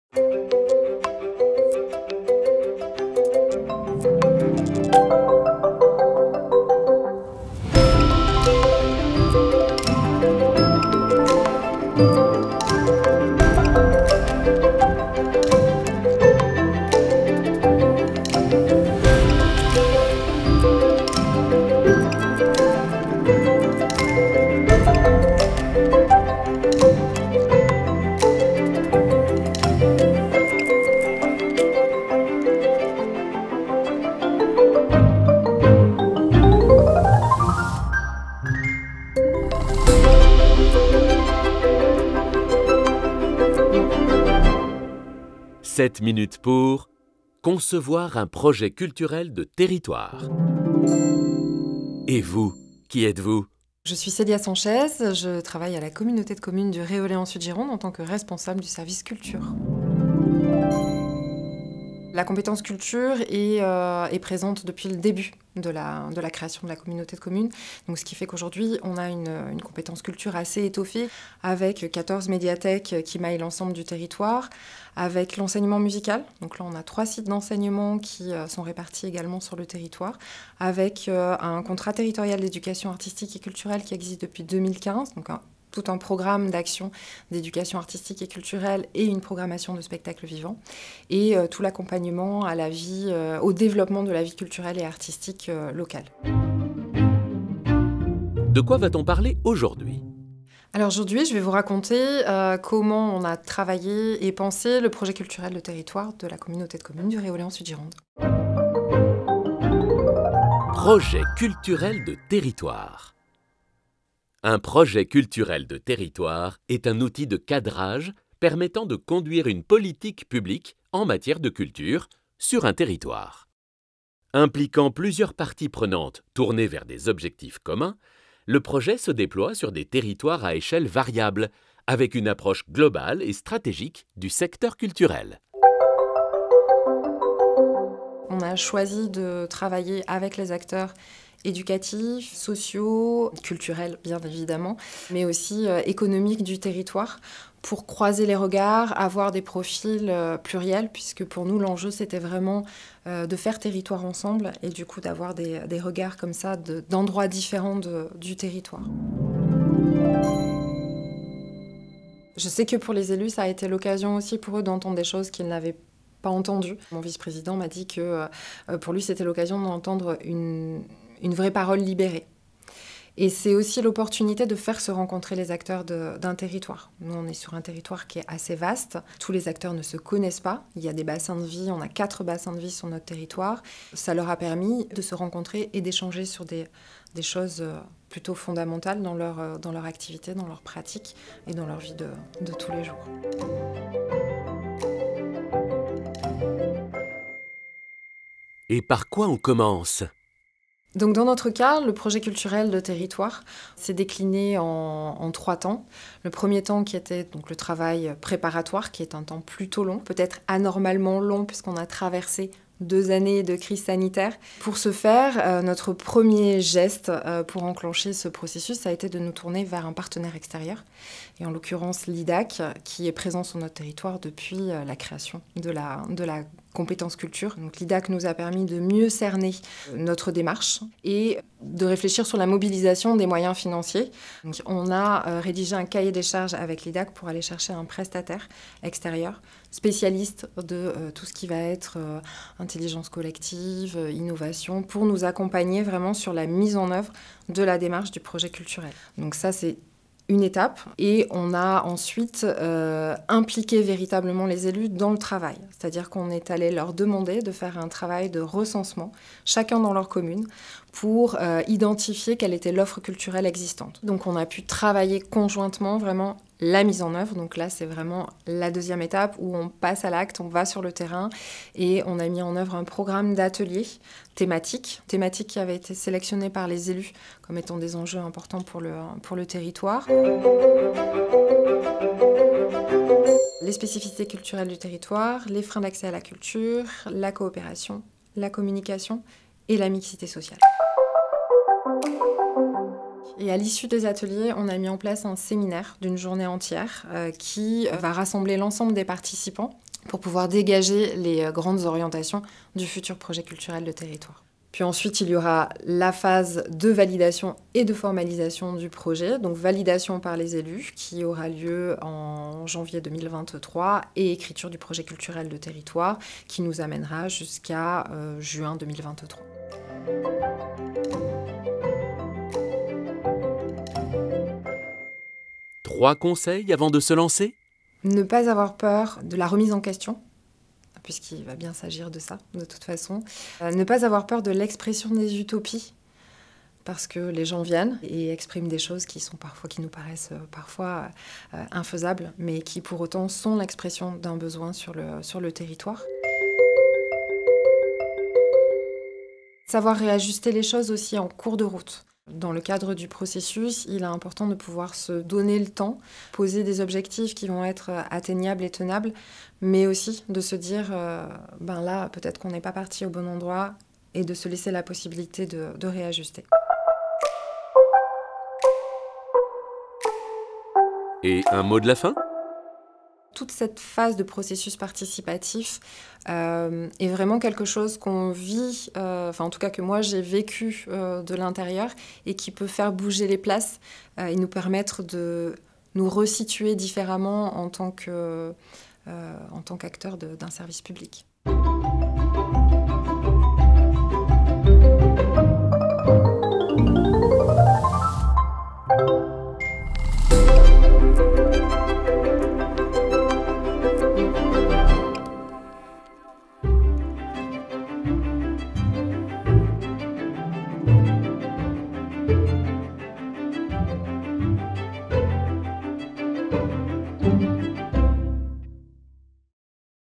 Concevoir un projet culturel de territoire avec Célia Sanchez
Une capsule de l’iddac avec Célia Sanchez, Communauté de
Communes du Réolais en Sud-Gironde. Réalisation : Les Embobinés, 2022
"7 minutes pour..." donne la parole à des expert·e·s et des porteur.euse.s de projets du territoire girondin. Une série de vidéos pensées en complémentarité avec les autres ressources proposées par l’iddac :
- pour aller + loin : Les Projets culturels de territoire, Emmanuel Négrier et Philippe Teillet, ouvrage disponible au Centre de Ressources iddac.